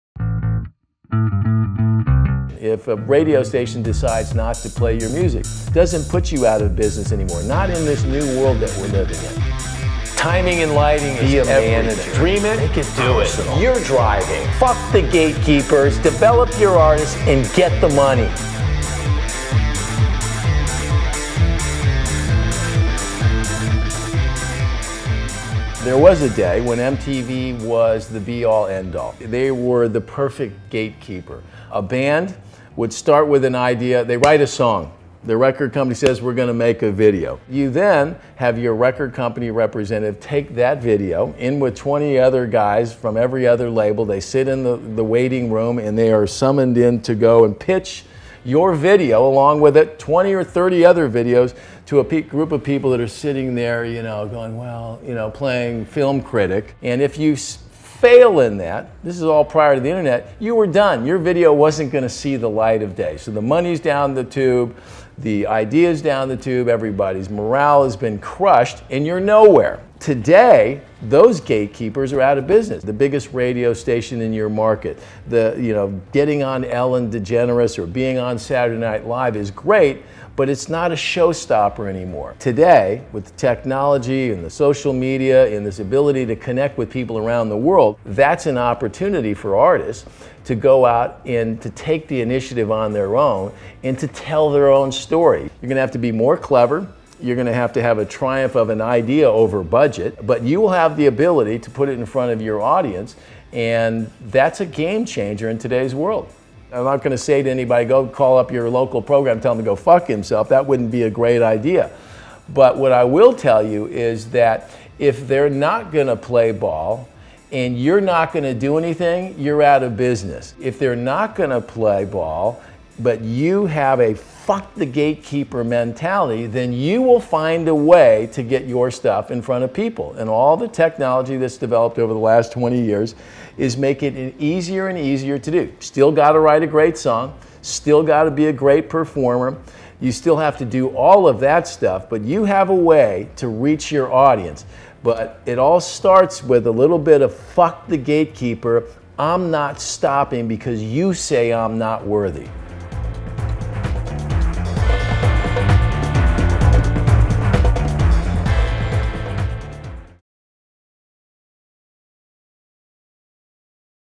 RMB Chapter 2: Fuck The Gatekeepers
aPpKVg

to sign up for a FREE DEMO of the Renman U course!

Before the internet, MTV was the be all end all of the music industry. They determined what videos, songs, and bands were "in" and if you weren't on that list you were never going to see the light of day. But in todays world the dream of turning an internet sensation into an actual career is starting to look more and more like reality. FTGK